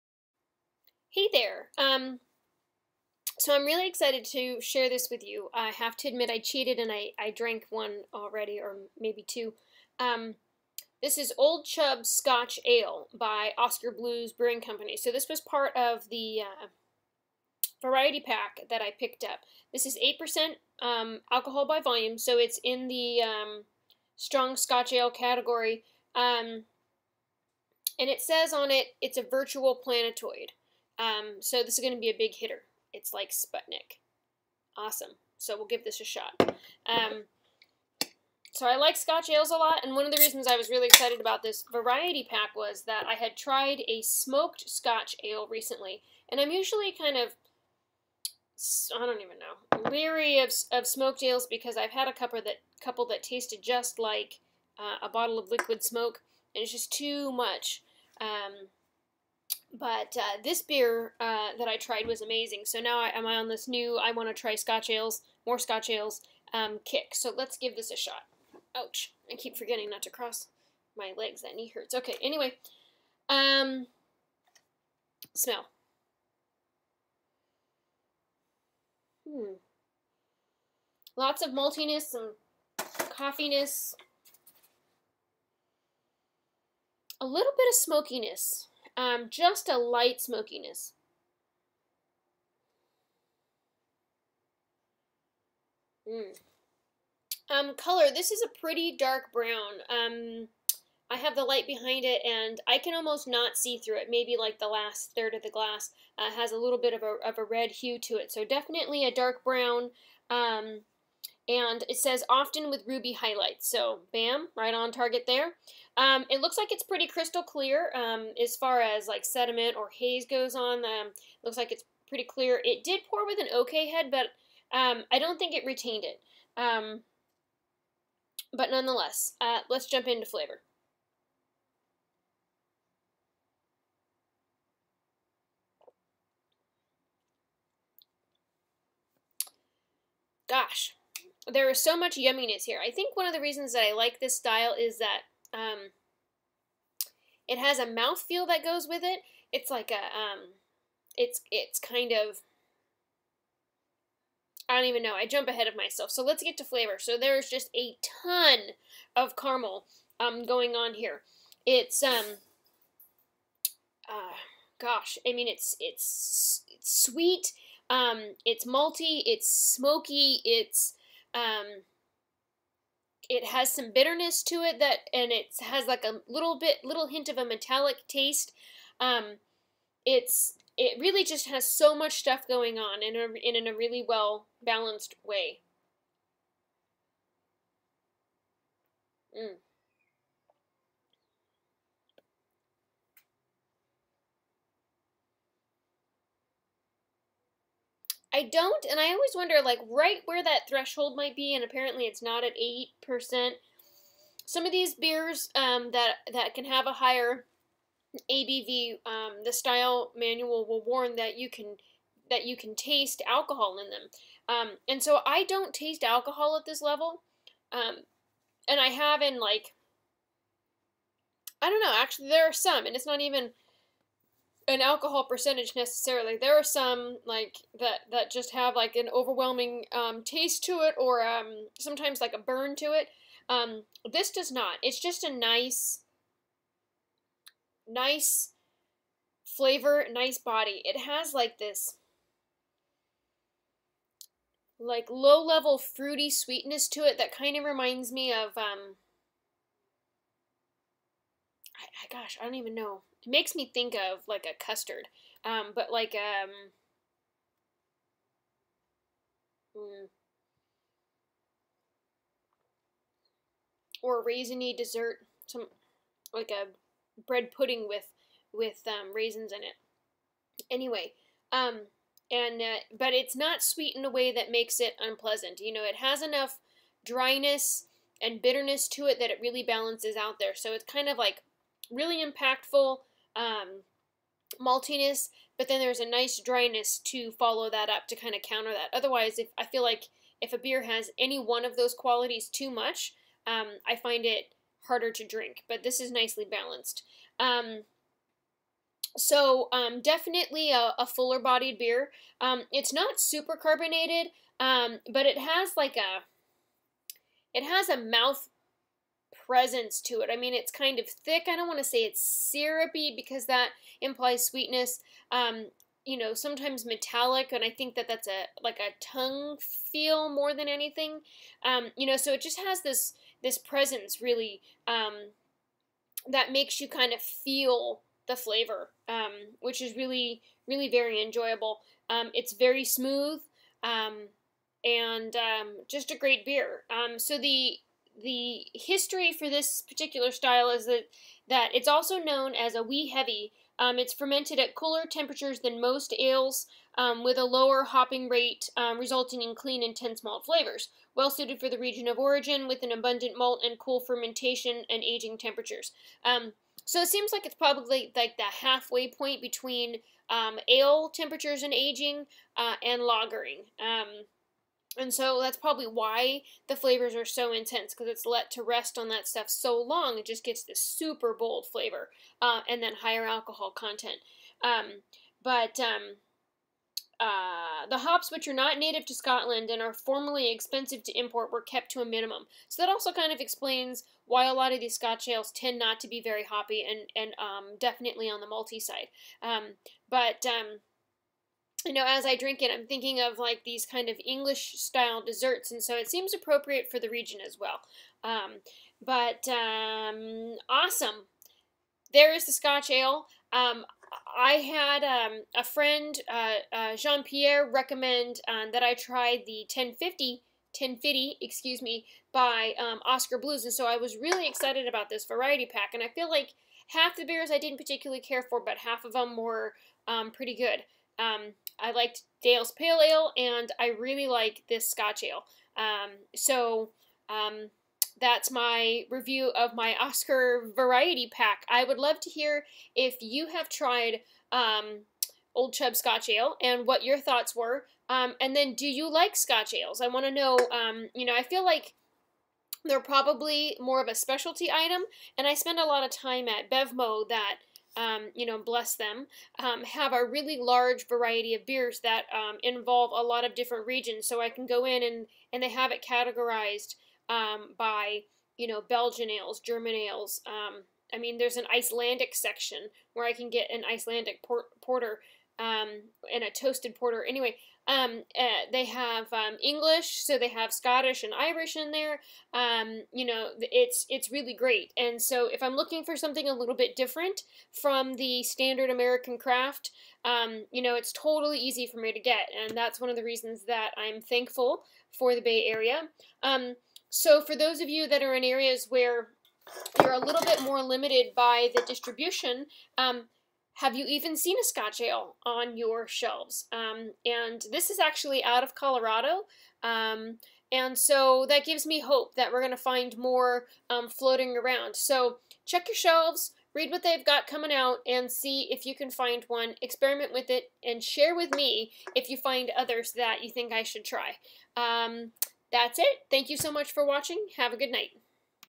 Oskar Blues Brewing Company Old Chub Scotch Ale - Scottish and Irish Ale, Strong Scoth Ale
I was excited to share this one!  Scotch Ales are such an enjoyable experience because they have modi and presence to go with the big malt profile.  This is sweet, bitter, fruiting, coffee-like and caramel-y all in one!  8% ABV.  It's in a can so take it to the pop with you!  I did!